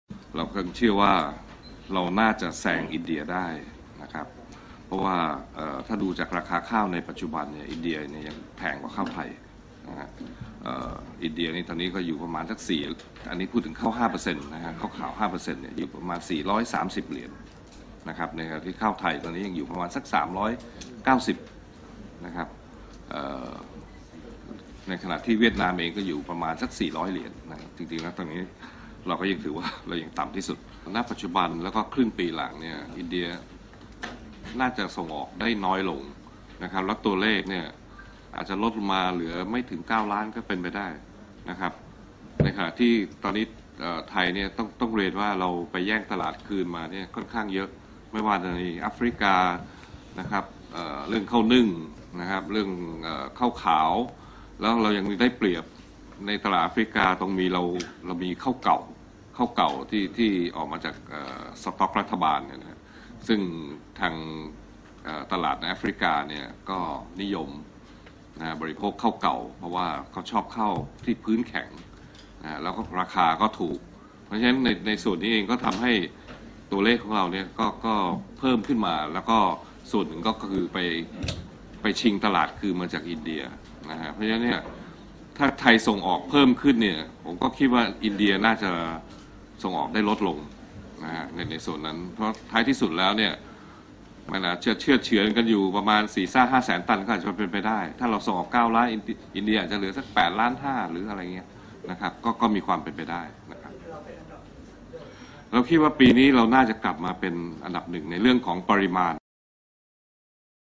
ไทยทวงแชมป์ส่งออกข้าวโลกได้สำเร็จ
ปีนี้ไทยผงาด กลับมาทวงคืนแชมป์ส่งออกข้าวโลกจากอินเดียได้สำเร็จ  คาด ทั้งปีจะส่งออกได้ 9 ล้านตัน